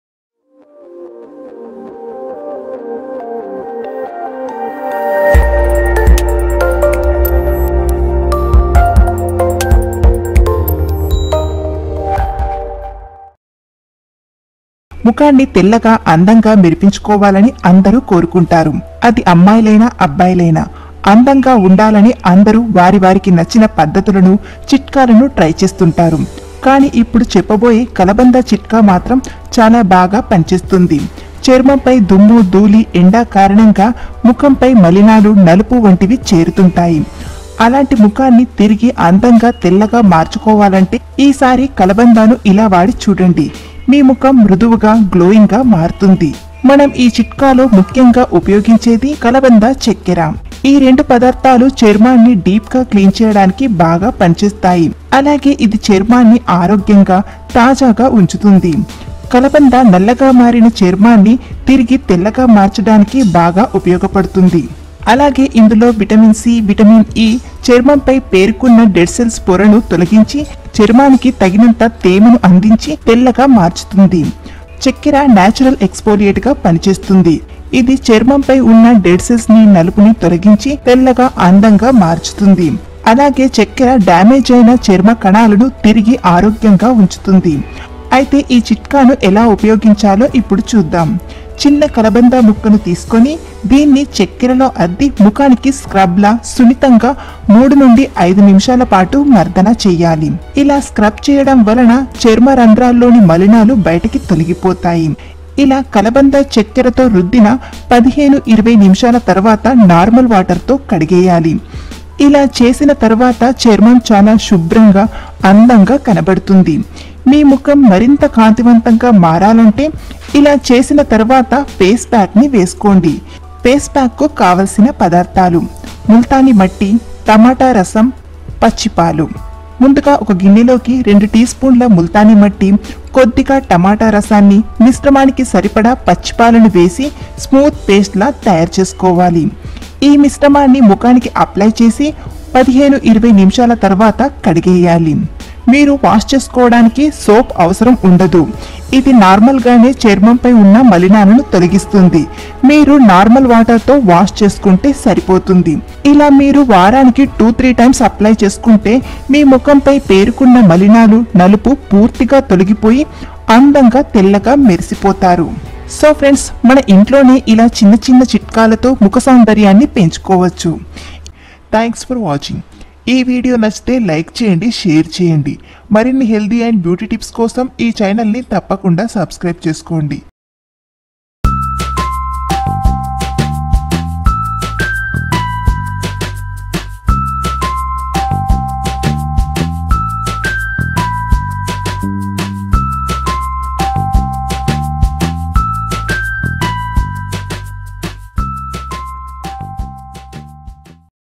🔴{TELUGU}Fairness Beauty Tips#Arogyasutralu
Fairness Beauty Tips||కలబందతో ఇలాచేస్తే ఎంత నల్లని ముఖమైనా తెల్లగా మెరిసిపోతుందిArogyasutralu